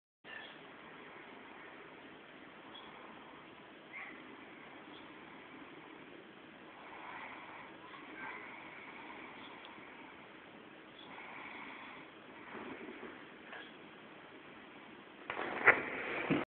Gurra snoring...sudden wake up! hilarious!
gusten is sleeping...and looks disoriented.